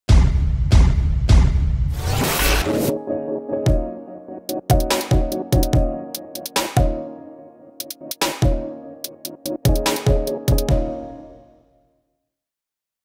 Don't Make Ads. Make TikToks
We have the resources your brand needs to authentically connect to culture and drive performance on TikTok. There's never been a better time to stop making ads and start making TikToks.